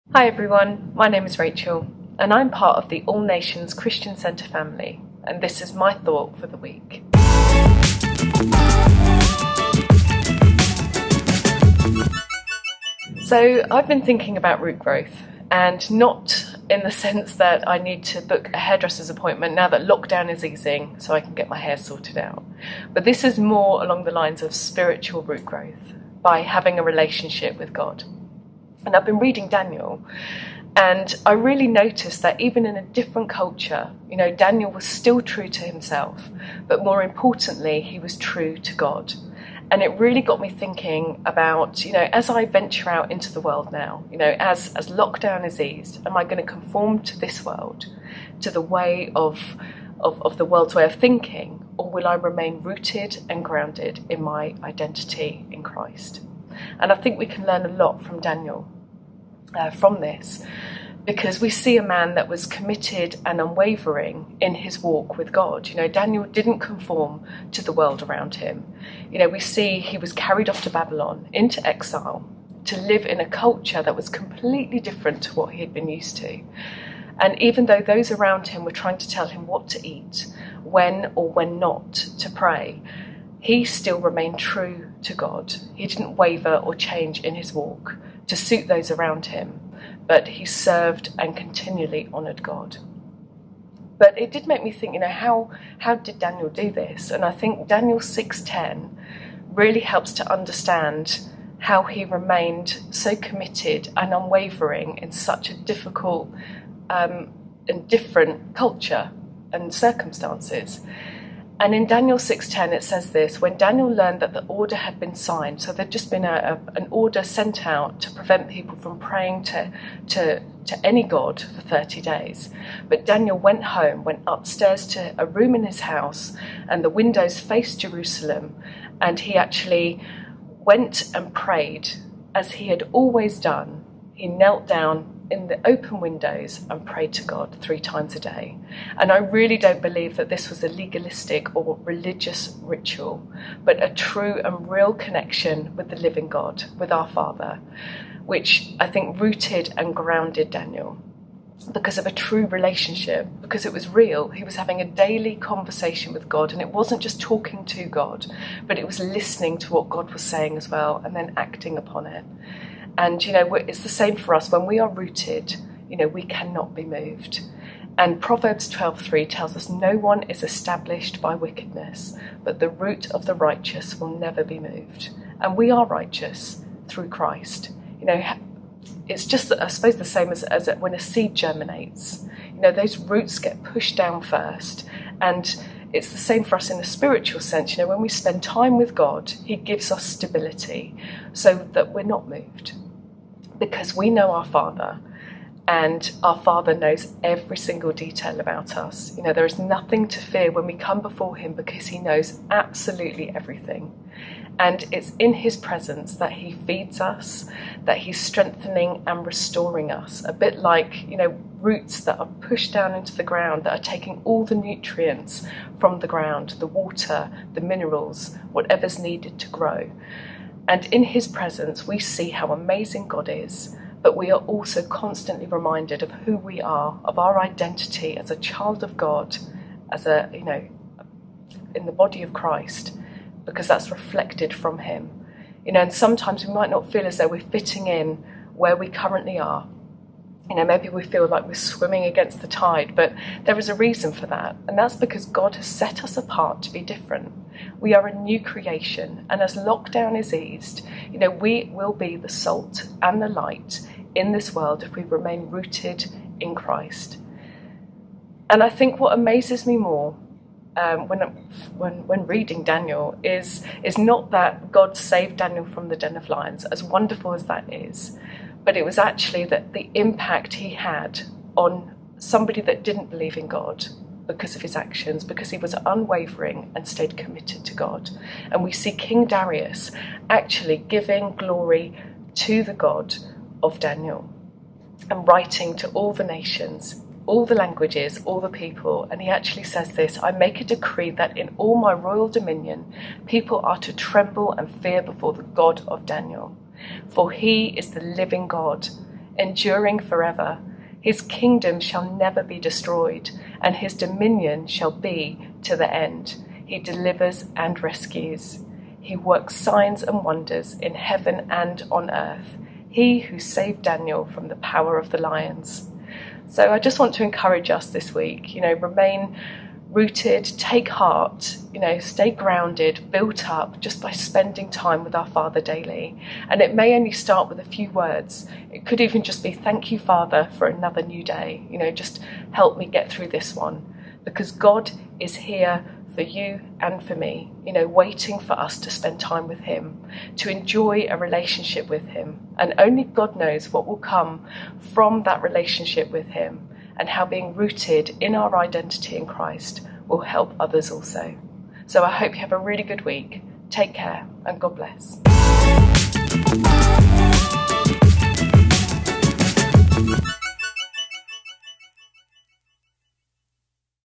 Thought For The Week - Root Growth
Rachel shares her thought for the week; root growth.

All Nations Christian Centre is an Elim Pentecostal Church in Reading, UK.